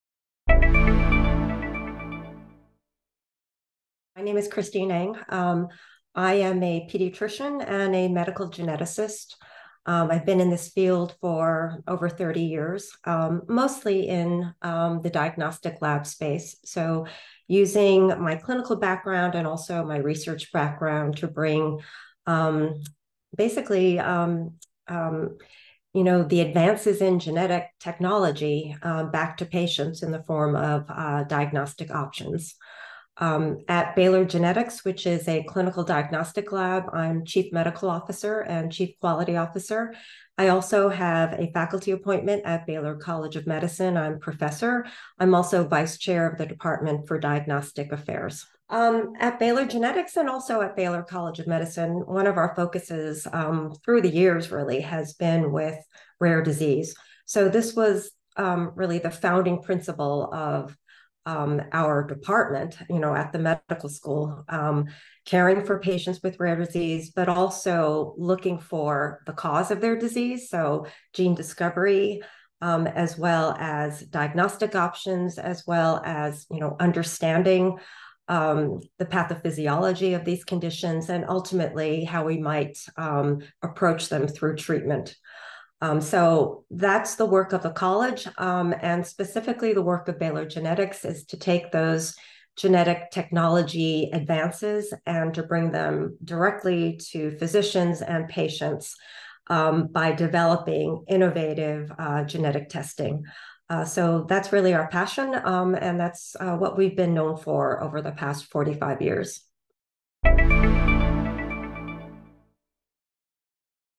Baylor Genetics
Christine Eng, Chief Medical Officer at Baylor Genetics diagnostic labs, explains Baylor Genetics and her involvement in the company.

Transcription:
My name is Christine Eng. I am a pediatrician and a medical geneticist. I've been in this field for over 30 years, mostly in the diagnostic lab space. Using my clinical background and my research background, to bring basically the advances in genetic technology back to patients in the form of diagnostic options. At Baylor Genetics, which is a clinical diagnostic lab, I'm Chief Medical Officer and Chief Quality Officer. I also have a faculty appointment at Baylor College of Medicine. I'm a professor. I'm also Vice Chair of the Department for Diagnostic Affairs.

At Baylor Genetics and at Baylor College of Medicine, one of our focuses through the years really has been rare diseases. This was really the founding principle of our department at the medical school. Caring for patients with rare diseases, but also looking for the cause of their disease. So, gene discovery, as well as diagnostic options, as well as understanding the path of physiology of these conditions, and ultimately how we might approach them through treatment.

That's the work of the college, and specifically the work of Baylor Genetics, is to take those genetic technology advances and to bring them directly to physicians and patients by developing innovative genetic testing. That's really our passion, and that's what we've been known for over the past 45 years.